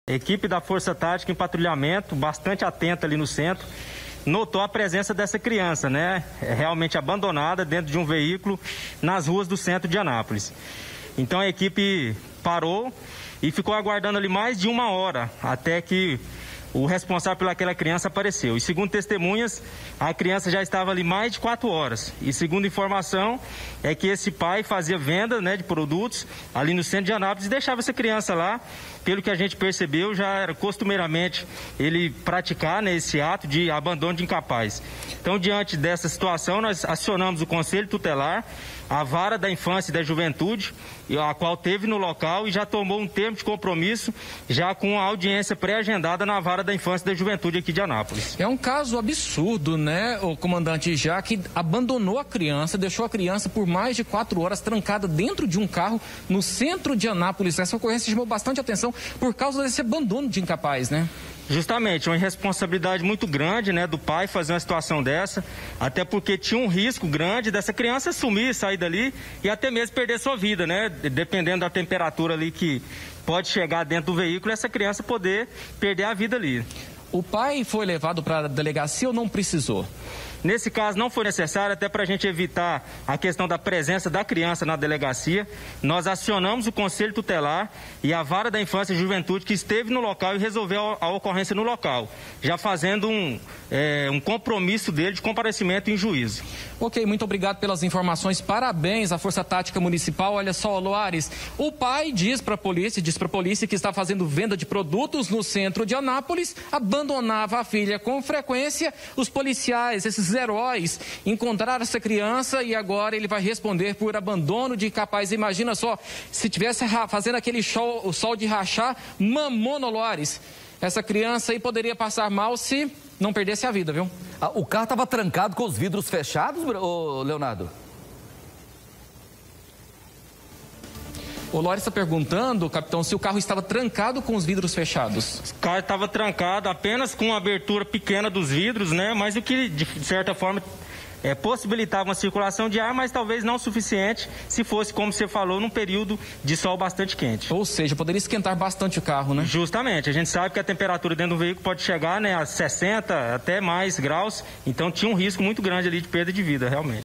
SUSTO: CRIANÇA É RETIRADA DE CARRO TRANCADO
UMA CRIANÇA DE CINCO ANOS FOI RETIRADA DE UM CARRO POR POLICIAIS MILITARES.